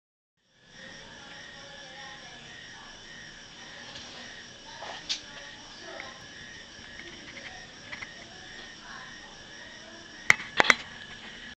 Leptodactylus latinasus
Pequeñas ranas urneras capturadas en Ucacha el 13 de octubre de 2008, camino a Bengolea en el camino por atrás al Monte San Juan